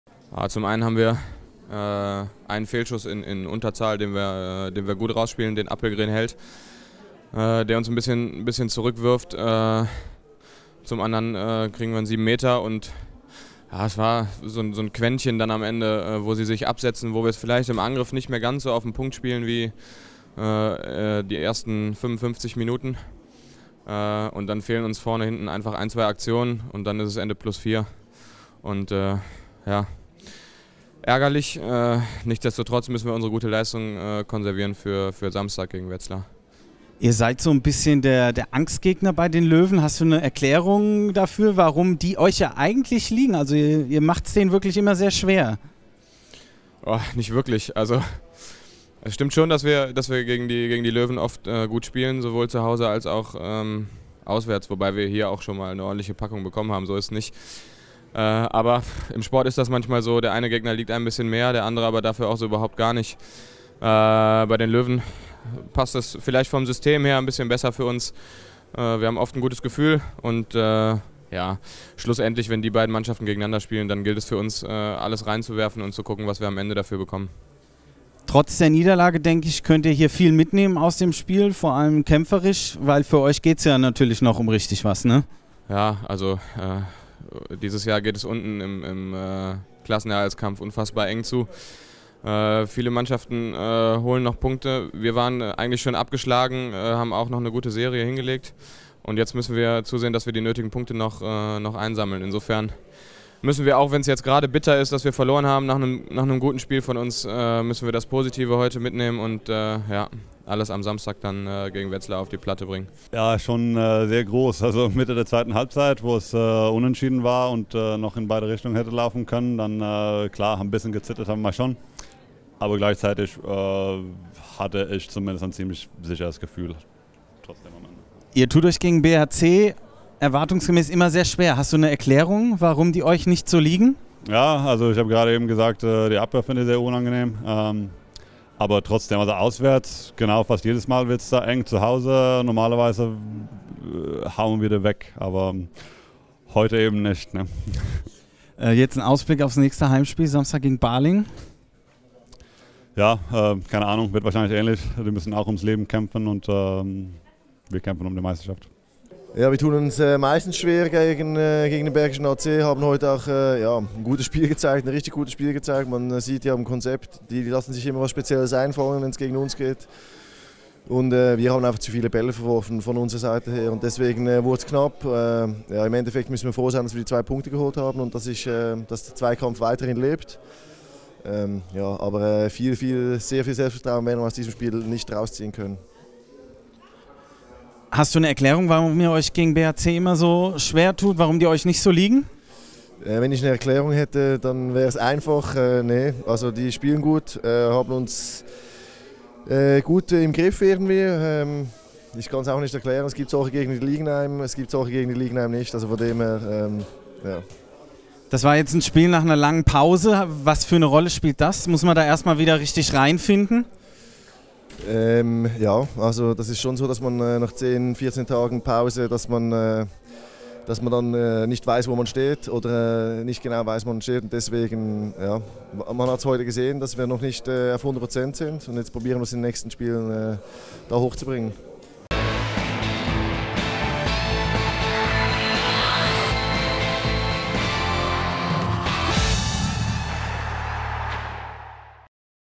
Interviews nach dem Löwen-Sieg gegen den Bergischen HC
Nach dem 31:28-Sieg der Rhein-Neckar Löwen gegen den Bergischen HC sprechen Christian Hosse vom BHC sowie die Löwen Kim Ekdahl du Rietz und Andy Schmid über die Partie.